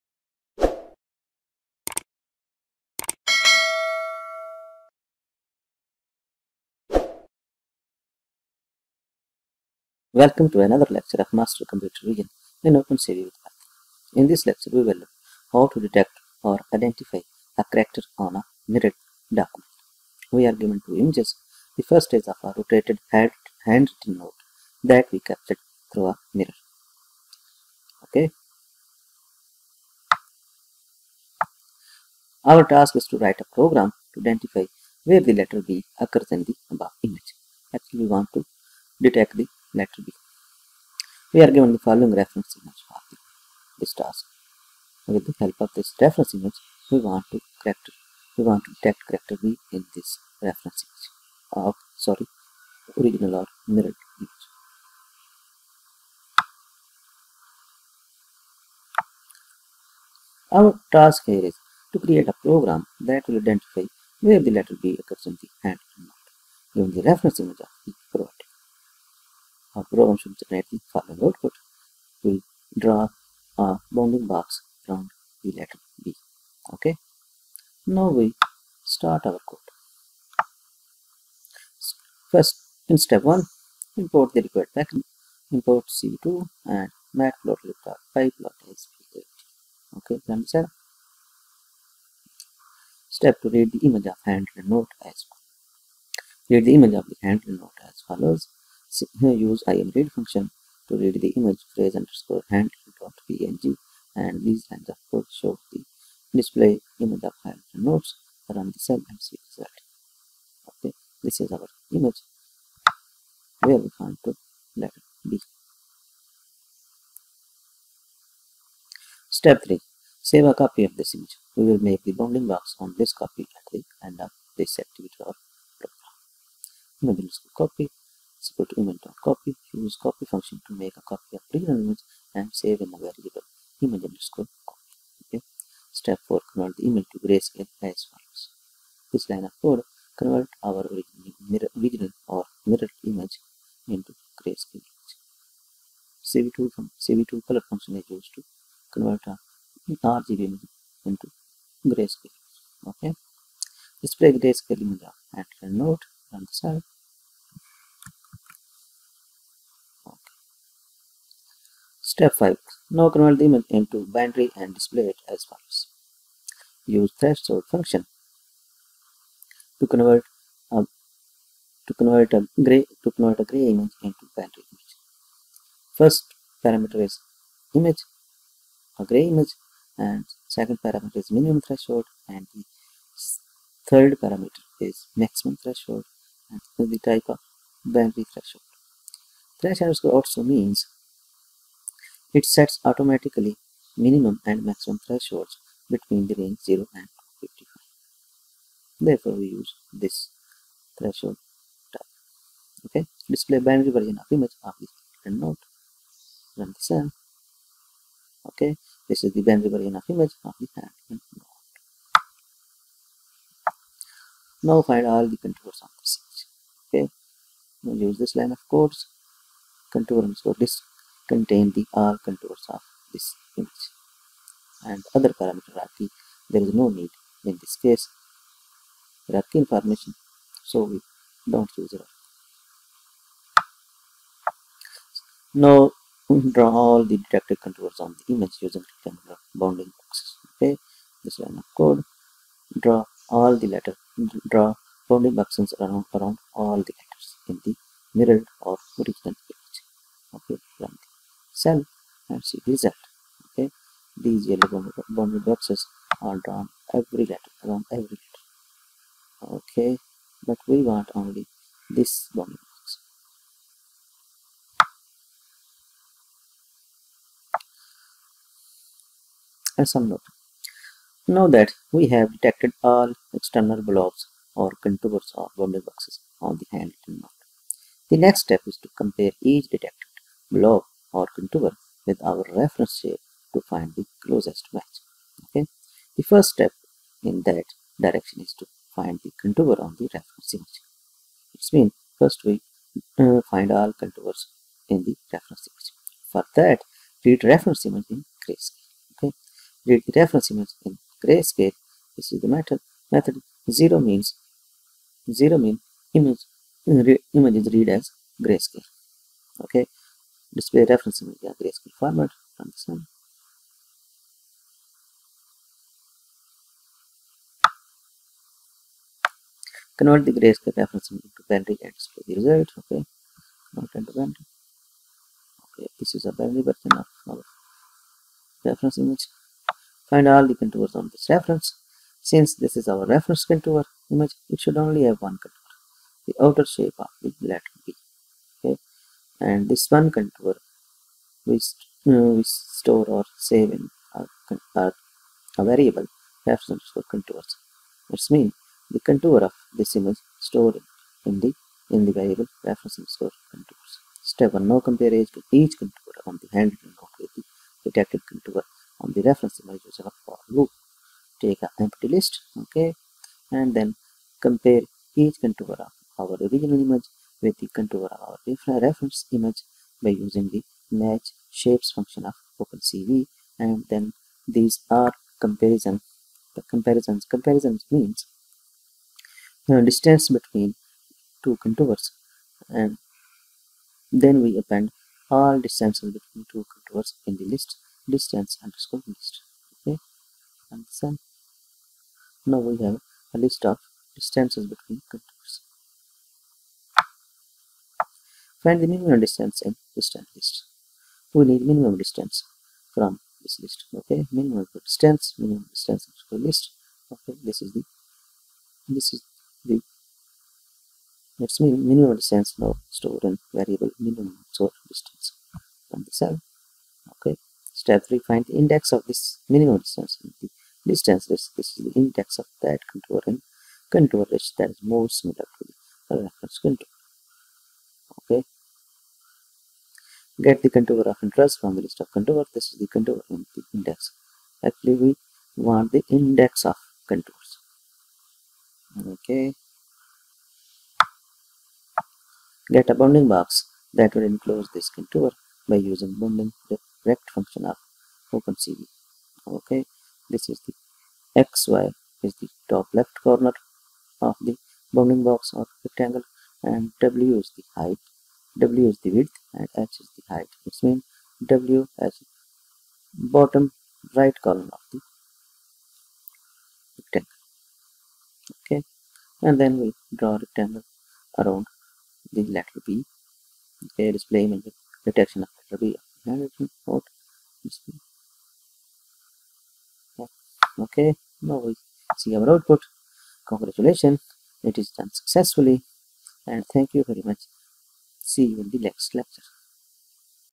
Detecting or Identifying a Character on a Mirrored Document
Detecting or Identifying a Character on a Mirrored Document. We are given two images. The first is of a rotated handwritten note that we captured through a mirror. Our task here is to create a program that will identify where the letter 'B' occurs in the handwritten note, given the reference image of 'B' provided.